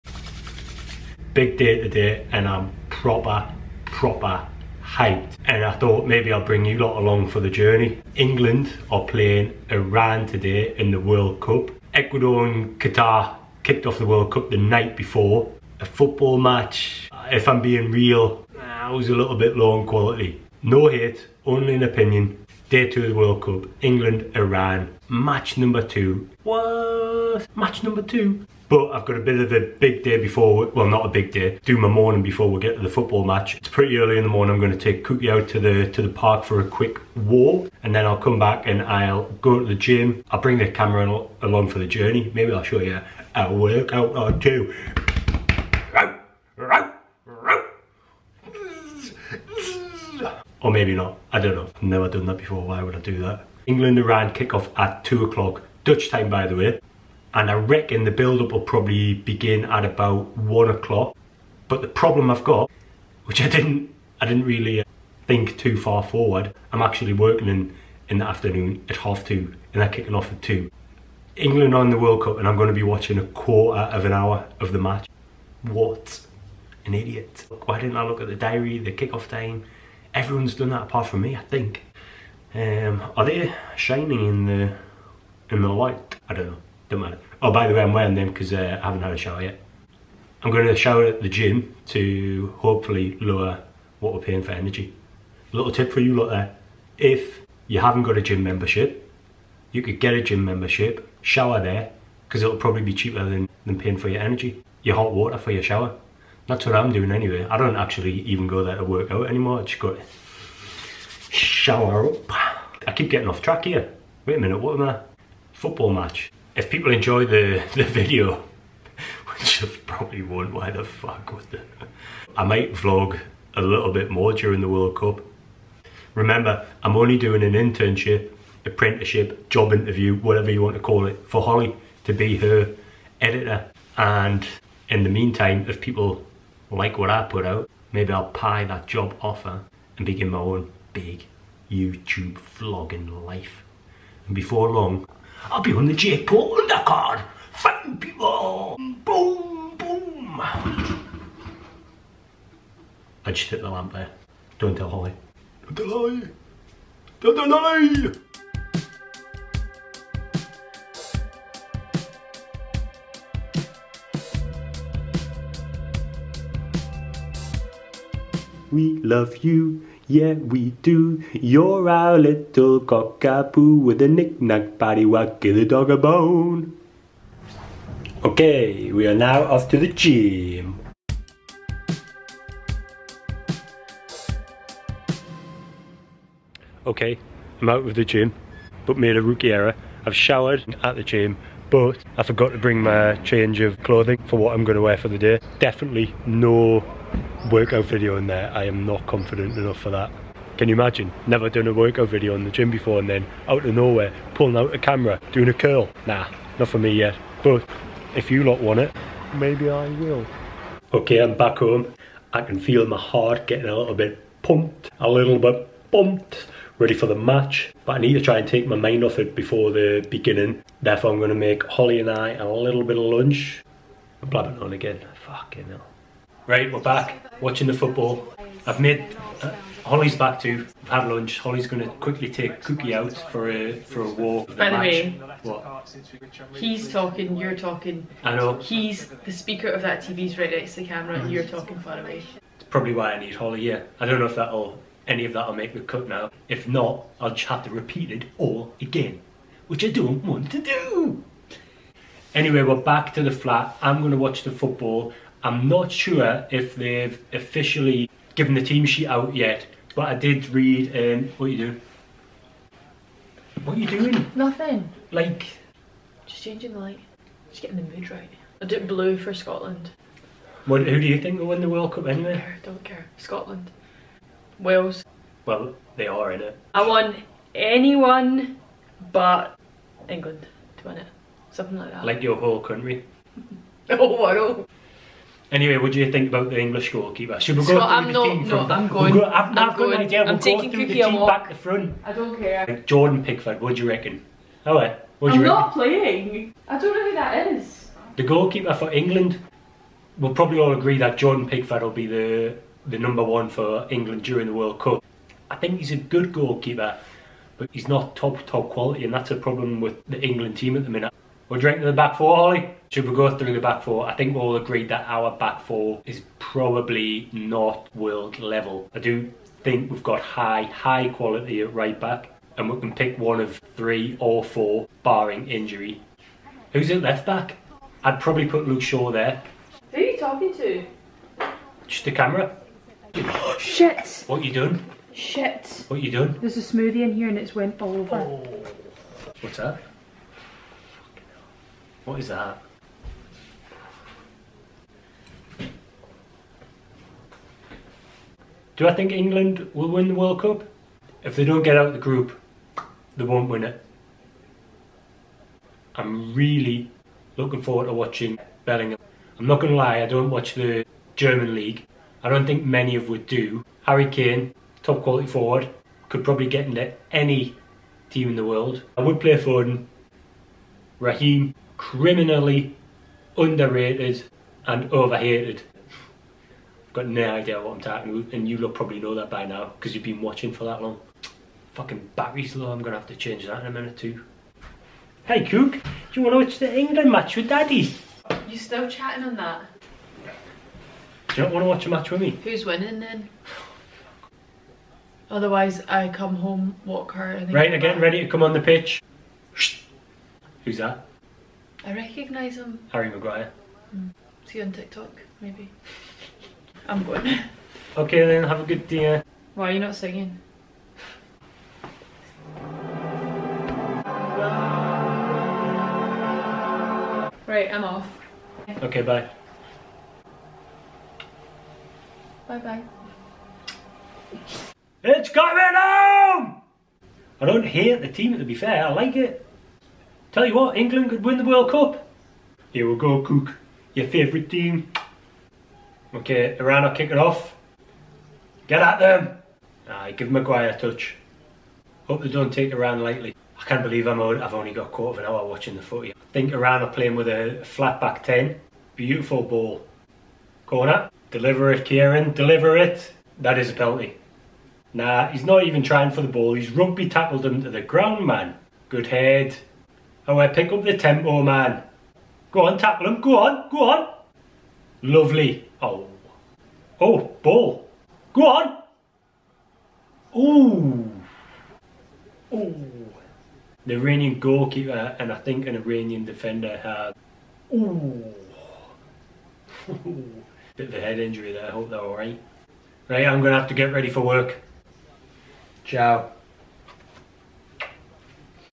ENGLAND vs IRAN World Cup vlog
Join me for a day in my life which includes walking my dog, going to the gym, watching the England vs Iran World Cup football match and then me leaving early to go to work.